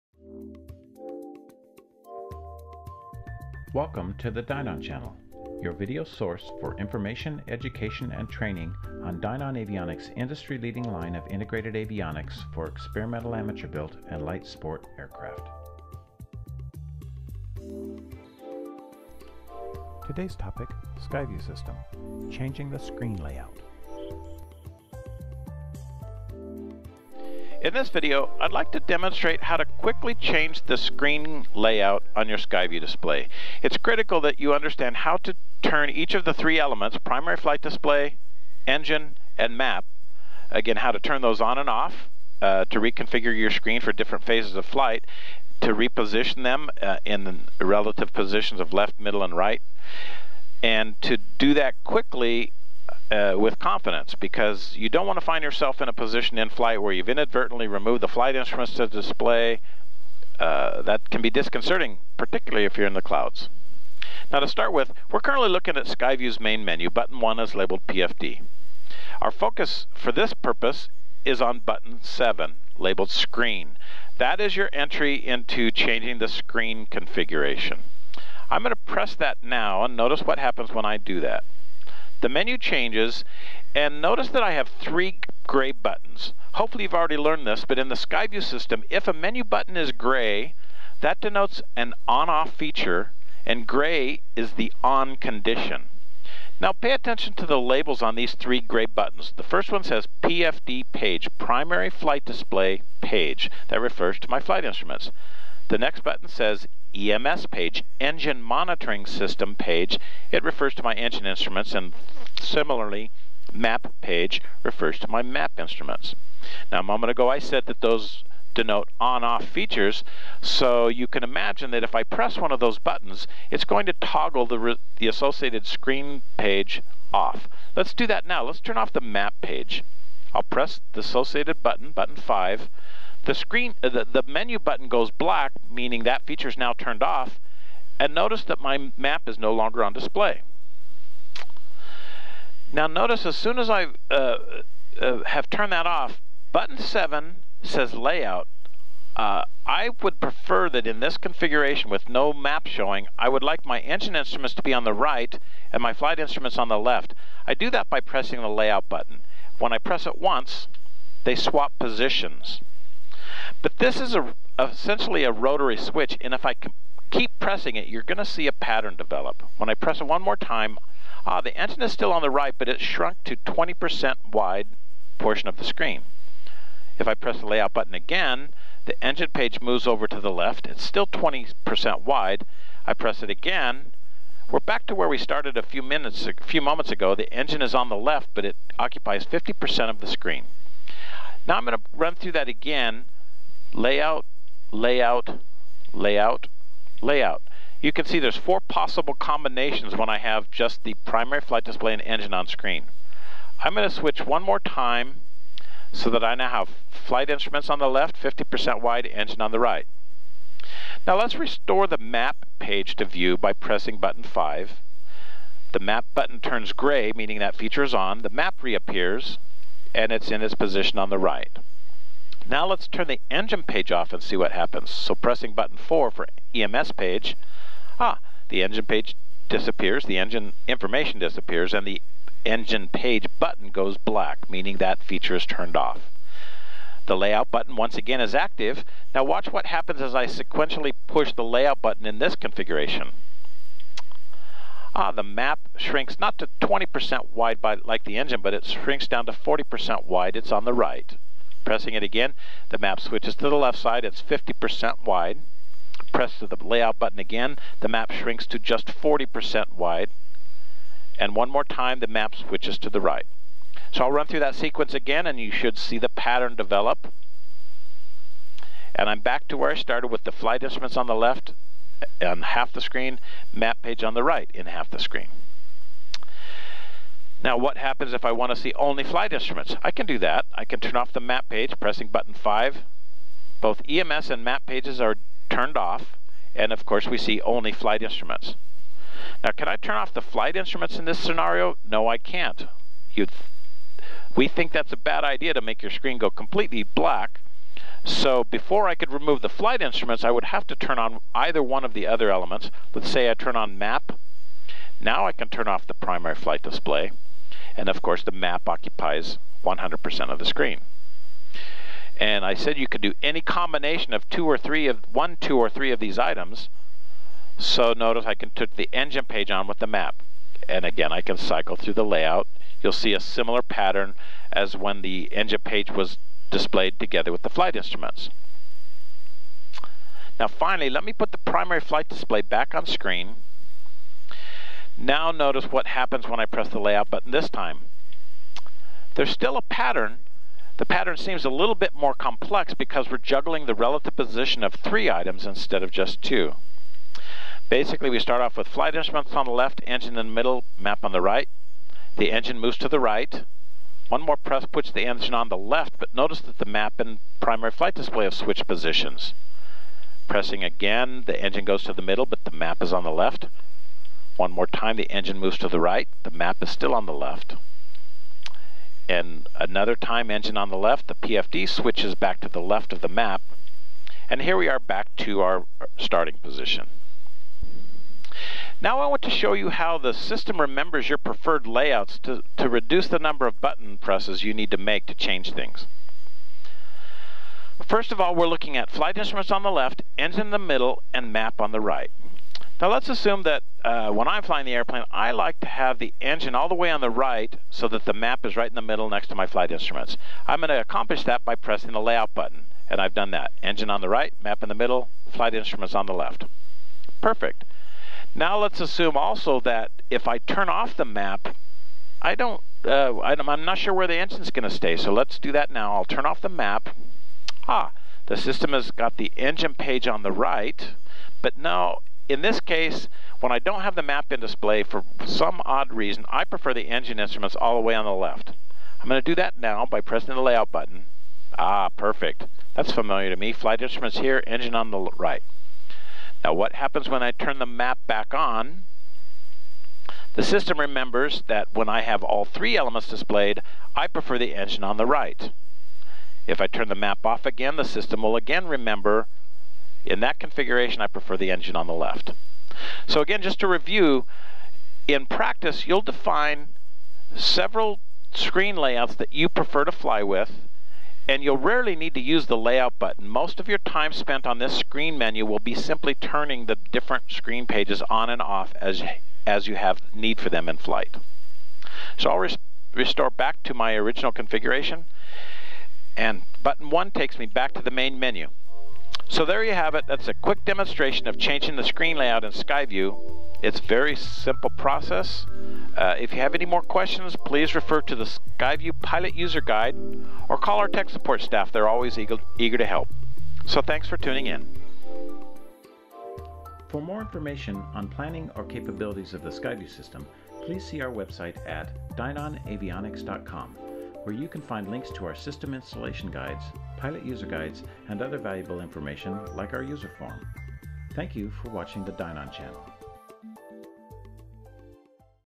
Changing the Screen Layout on a SkyView Display
Discover how easily the SkyView display's layout can be changed to show any combination of flight instruments, engine instruments, and moving map.